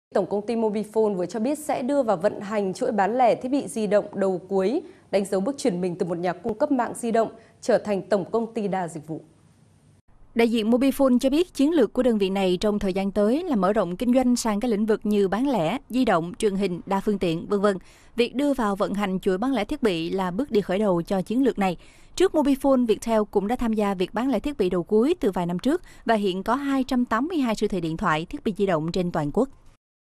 FBNC - Mobifone sắp ra mắt chuỗi bán lẻ thiết bị di động
Tổng công ty Mobifone vừa cho biết sẽ đưa vào vận hành chuỗi bán lẻ thiết bị di động đầu cuối, đánh dấu bước chuyển mình từ một nhà cung cấp mạng di động trở thành tổng công ty đa dịch vụ.